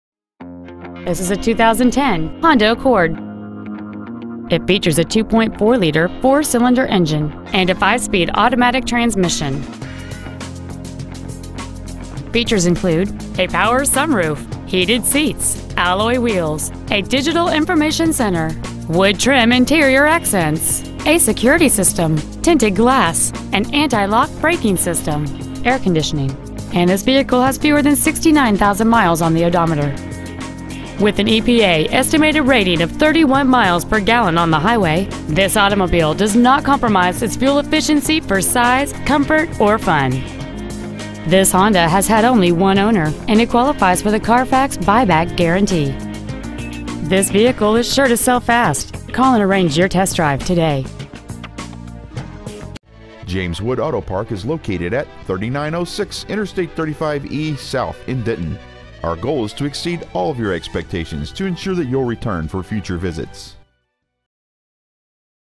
2010 Honda Accord Ft. Worth TX
Year : 2010
 Make : Honda
 Model : Accord
 Engine : 2.4L 4 Cyl. Sequential-Port F.I.
 Trans . : 5 Speed Automatic
 Exterior : Basque Red Pearl
 Miles : 68,930

 Stock : 223609A1

 Used 2010 Honda Accord Denton TX